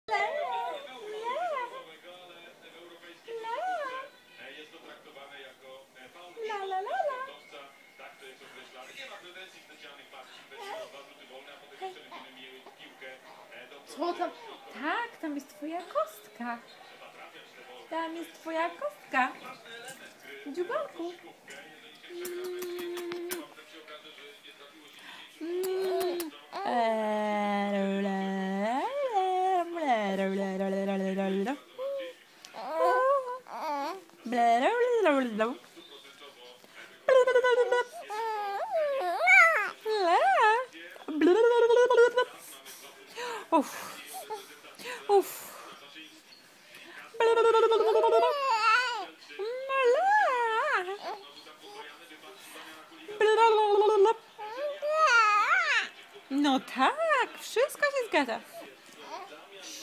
Hania konwersuje z mamą !
9 Września 2015 - Mieszkanie na Janickiego